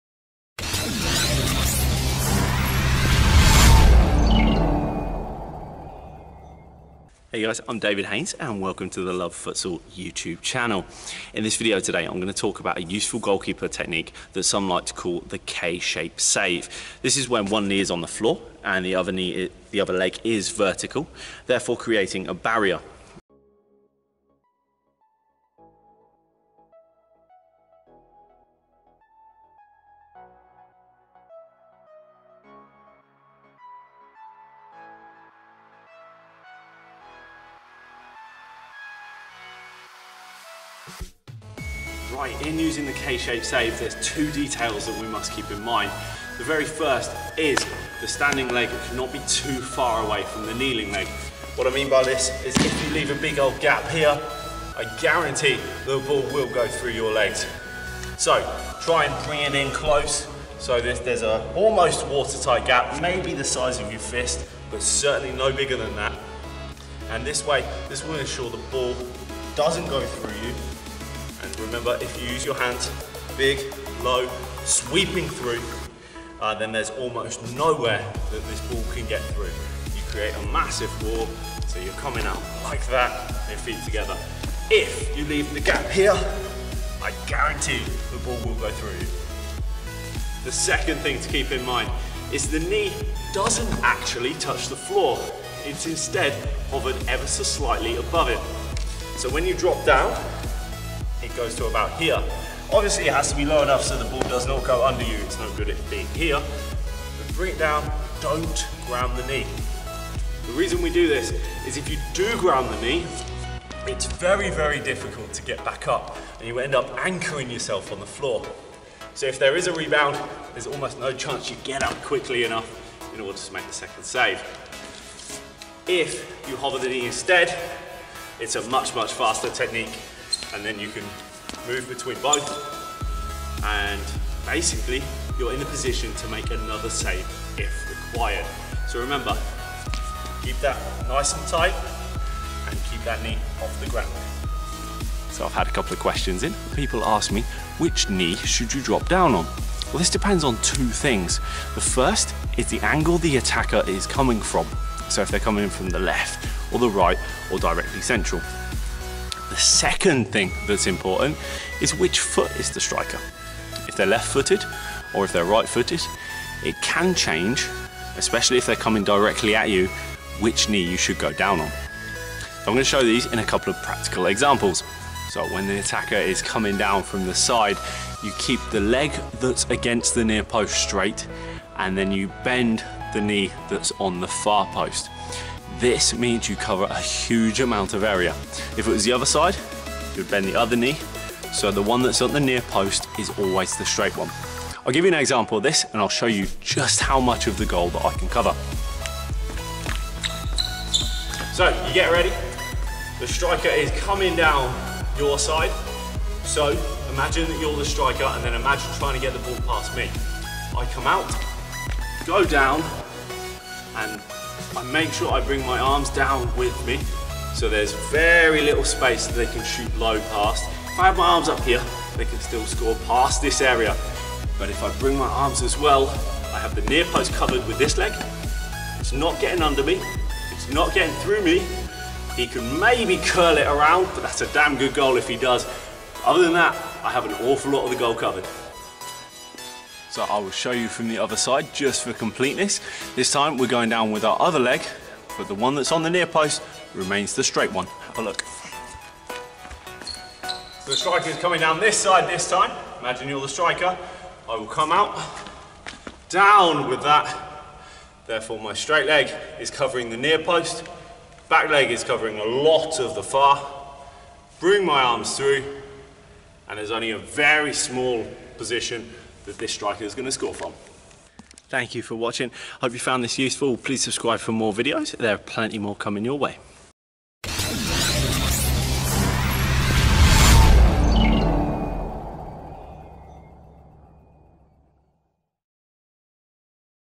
Learn how to do THE FUTSAL BLOCK SAVE! Tips, Advice and Tutorial!
Tips and advice on the Futsal Block Save.

Bahasa Indonesian SUBTITLES ADDED! Click Bottom Left of the video.